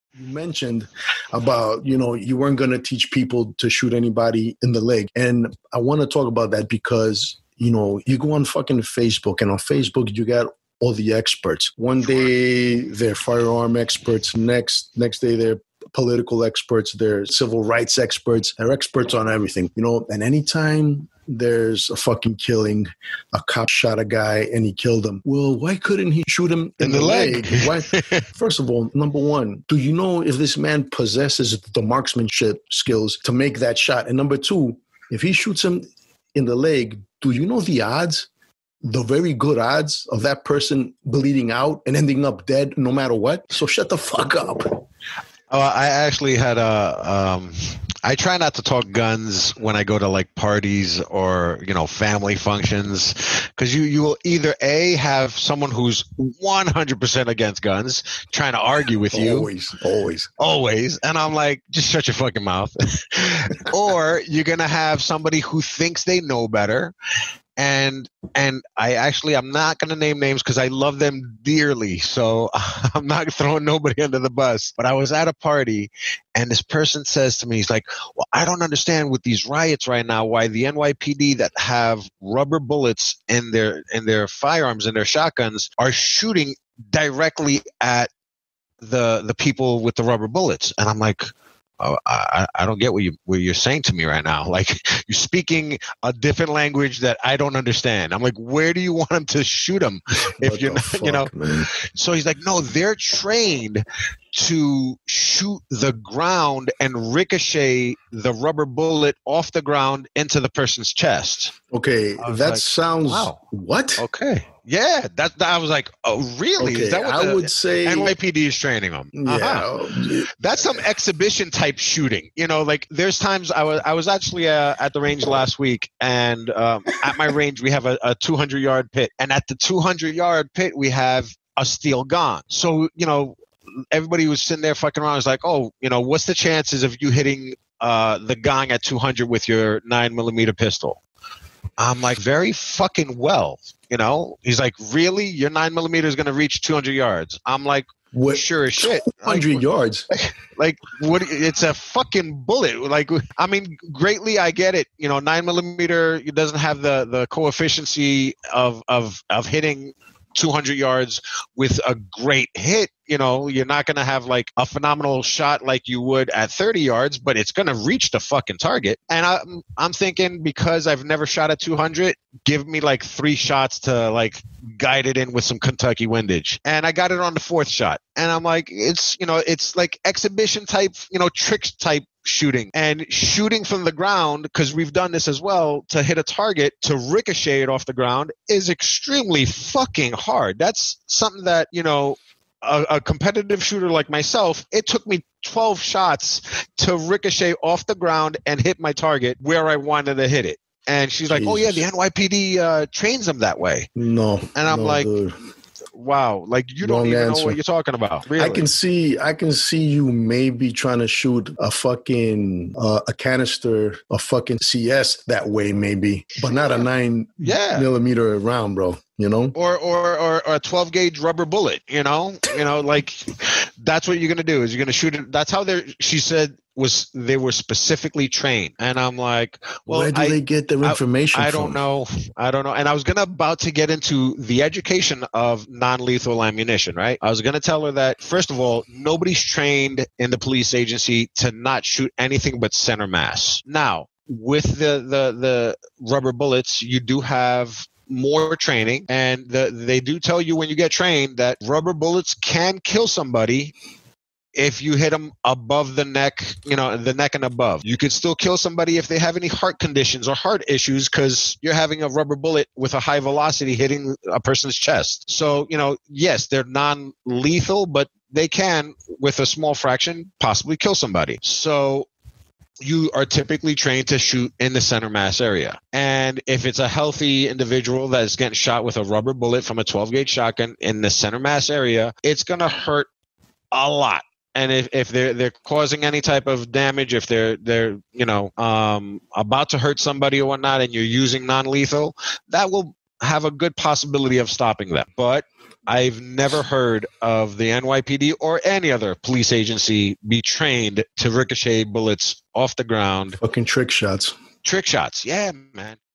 Rubber Bullet Trick Shots
Spiro and professional competitive gun shooter, Tommy Georgiopoulos, react to the revelation by keyboard experts who claim that the NYPD is trained in exhibition trick shooting.